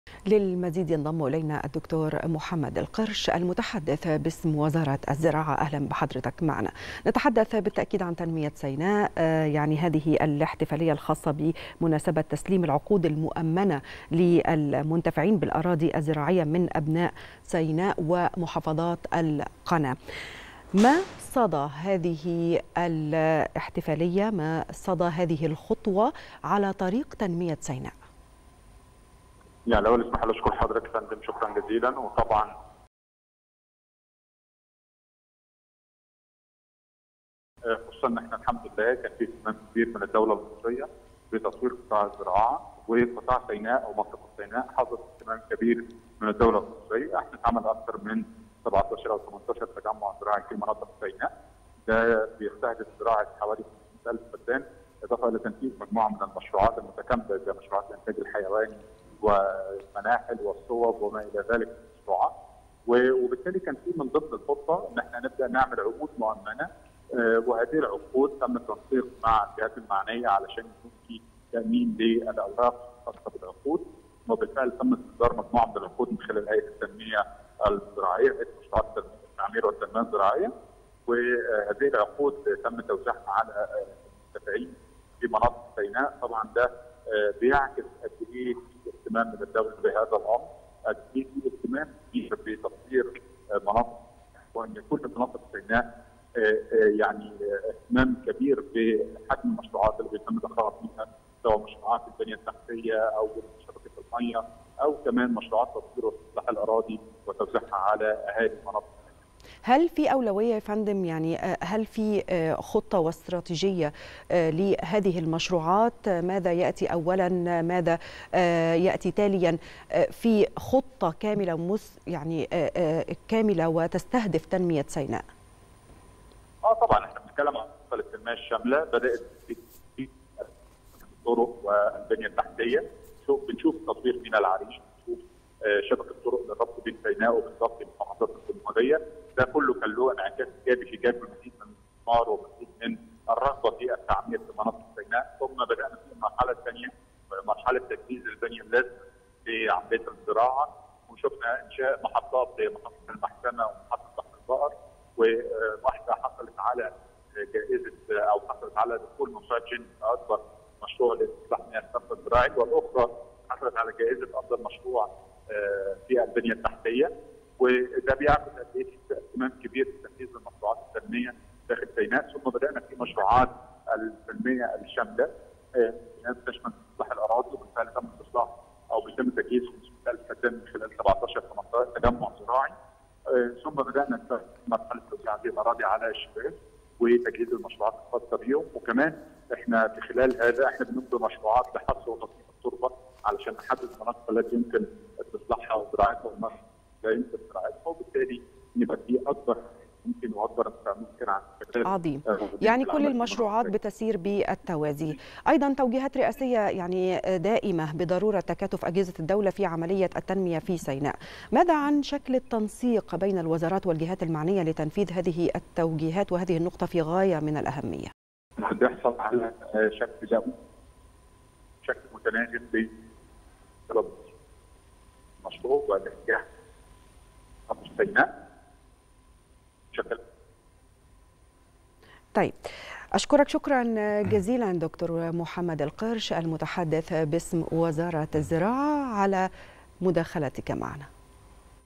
المتحدث باسم وزارة الزراعة: اهتمام كبير من الدولة المصرية بتطوير قطاع الزراعة وفي سيناء بشكل خاص
نشرة الثالثة | د. محمد القرش المتحدث باسم وزارة الزراعة: اهتمام كبير من الدولة المصرية بتطوير قطاع الزراعة وفي سيناء بشكل خاص